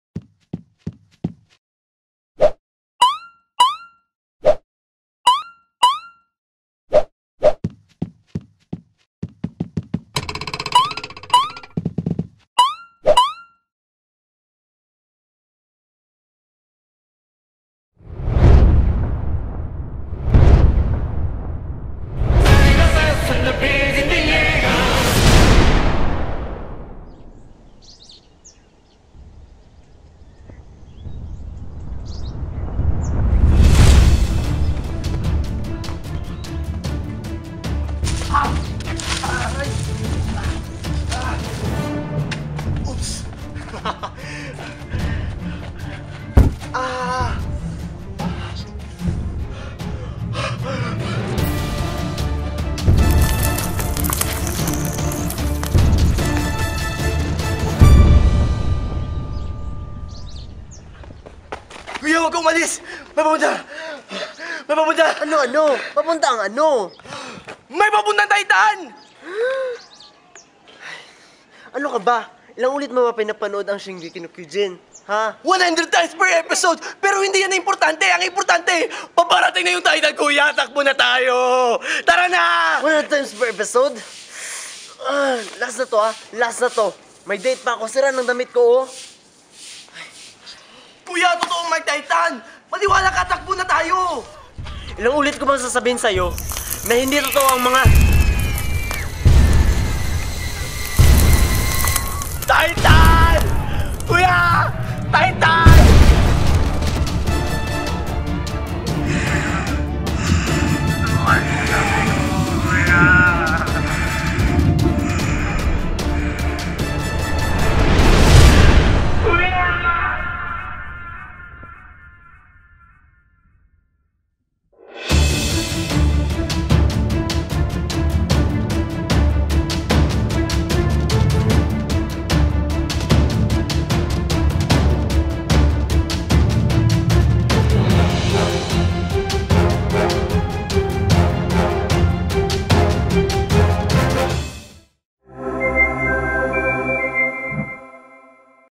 TITAN! - Animé Addicts Ep. 1
Two brothers has the power to make animé elements real! What happens when they make a titan from Attack on Titan real? O.O

Wanna know how we make awesome videos?

GulliermoGage

You an animator? Want to join a network like me?
Try joining Channel Federator Network!